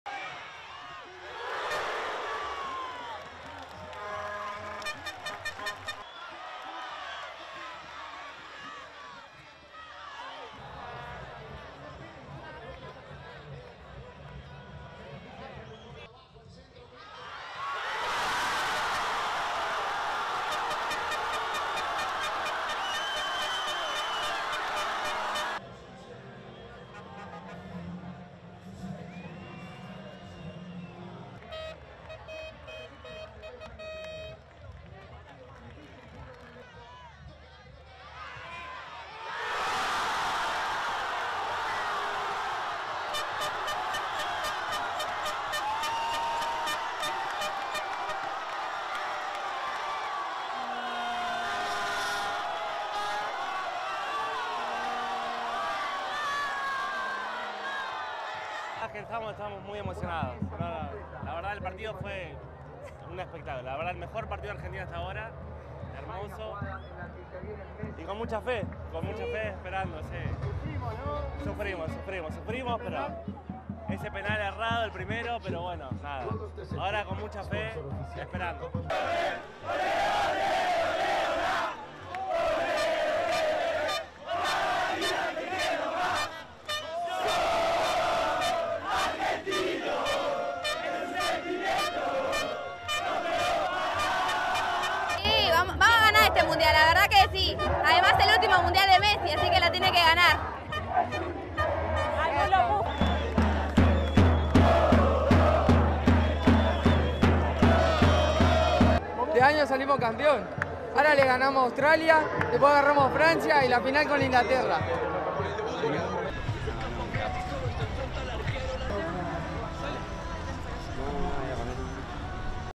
Fans in Argentina watch Poland match on big screen
RESTRICTION SUMMARY:

ASSOCIATED PRESS - AP AND SNTV CLIENTS MAY USE
Buenos Aires, Argentina - 30 November 2022
1. Various of people watching game
2. A woman reacts to Lionel Messi's failed penalty kick
3. Fans react to first goal by Argentina's Alexis Mac Allister
4. Fans open a flag with images of late soccer star Maradona and Lionel Messi
5. Fans, one wearing mask that resembles Messi
6. Fans react to goal by Argentina's Julian Alvarez
7. SOUNDBITE (Spanish): Roque Cloister, left, and his girlfriend Daria Kostets:
"We are very moved, the match was a show, the best match of Argentina so far, beautiful and with a lot of faith, waiting. We suffered with that missed penalty but nothing, now with a lot of hope and waiting."
8. Fans chanting UPSOUND (Spanish) "Everyday I love you more, I'm Argentine, it's a feeling I can't stop."
9. SOUNDBITE (Spanish): Camila Torres, 18, fan:
"We will win this World Cup, and this is Messi's last World Cup so he has to win it!"
10. Fans cheering
11. SOUNDBITE (Spanish): Diego Martinez, fan:
"This year we will be champions, first we will beat Australia, then France and the final against England."
12. Fan posing by photo of Messi
STORYLINE:
Hundreds of fans watched Argentina beat Poland on a giant screen in a park in Buenos Aires.

Lionel Messi had a penalty saved but his team still beat Poland 2-0 Wednesday after second-half goals from Alexis Mac Allister and Julian Alvarez, and advanced to the last 16.

After opening the World Cup with a shocking 2-1 loss to Saudi Arabia in one of biggest upsets in the tournament’s history, Argentina wound up finishing in first place in Group C and will next play Australia — a surprise qualifier for the knockout stage.

AP Video by Victor Calvano